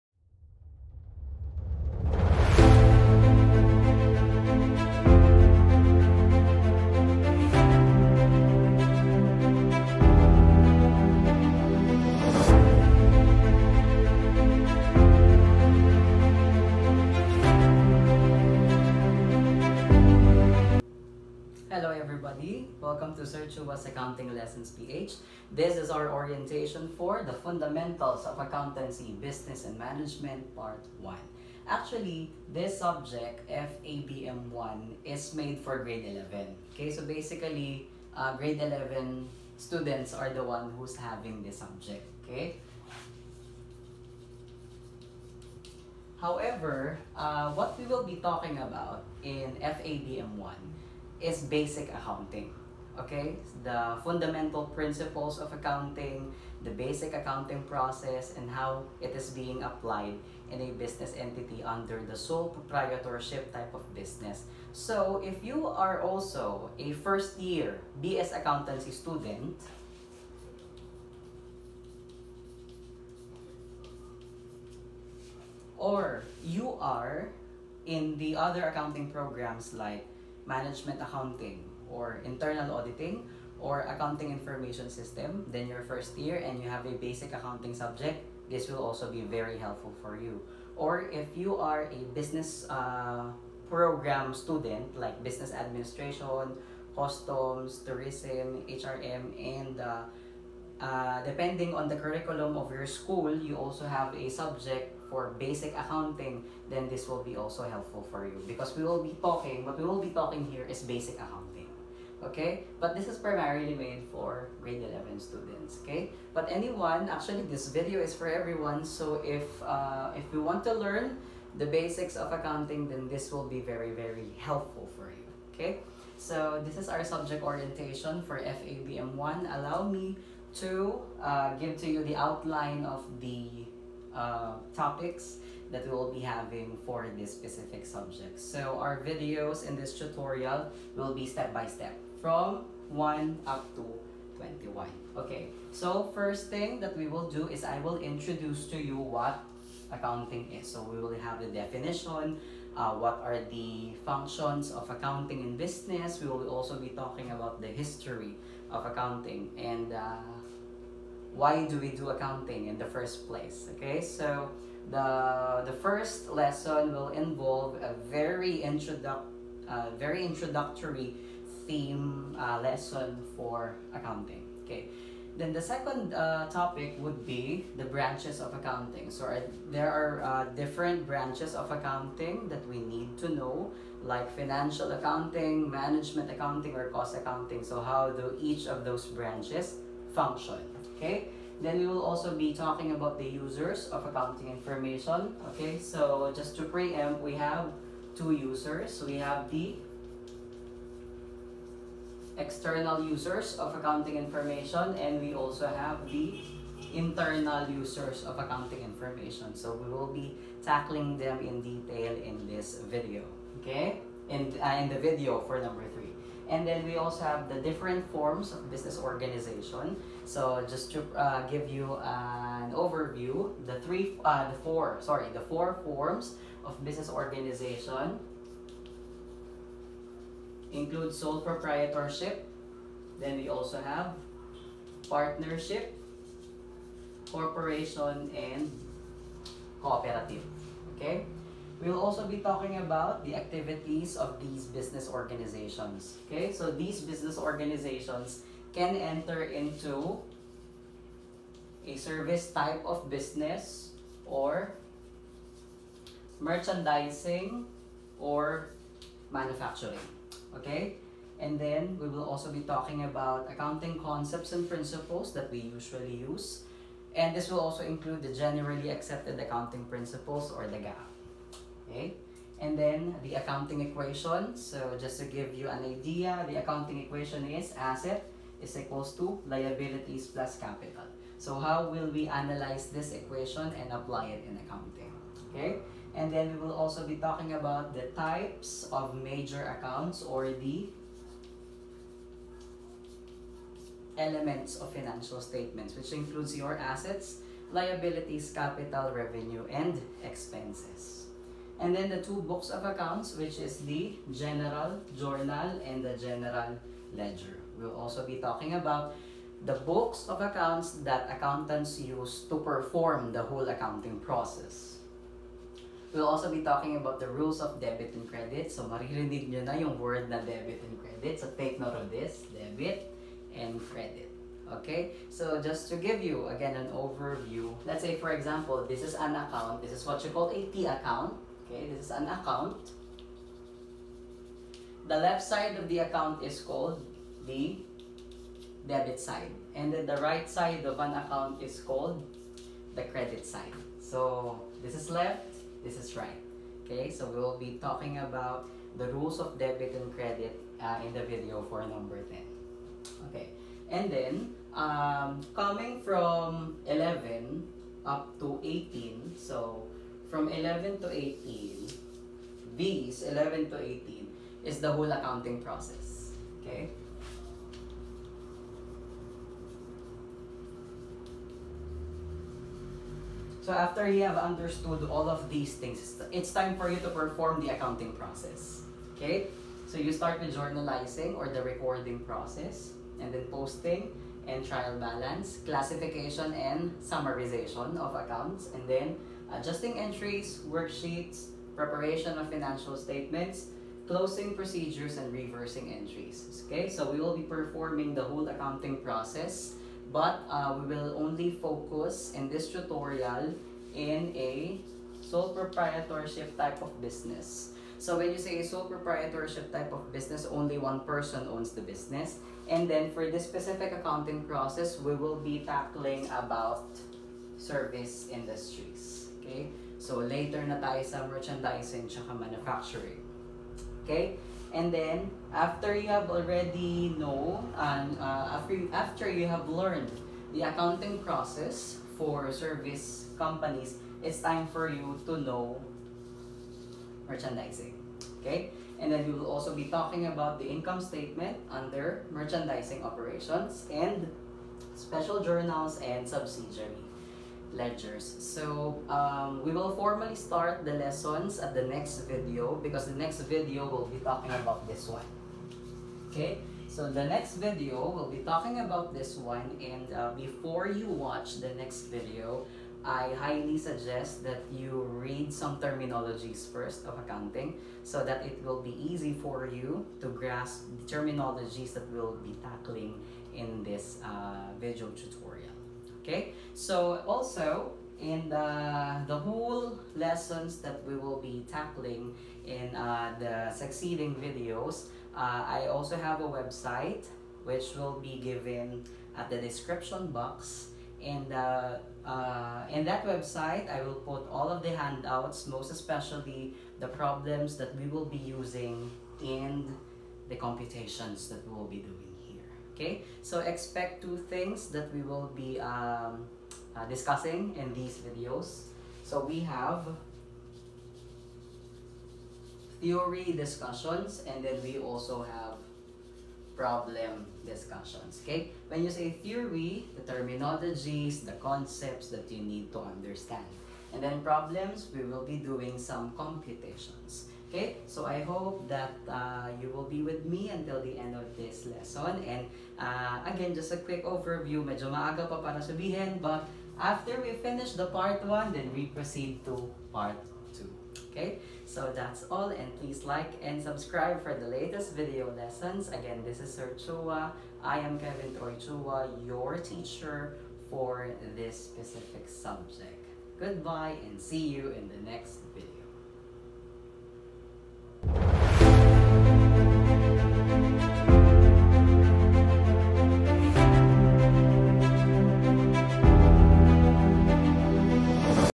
Lesson 002 - Subject Orientation: Fundamentals of Accountancy, Business, and Management 1
Hi! This is Sir Chua's Accounting Lessons PH

Lesson 002
Subject Orientation: Fundamentals of Accountancy, Business, and Management 1

For comments, suggestions, questions, and reactions, please comment them down below. Please make your comment as respectful as possible.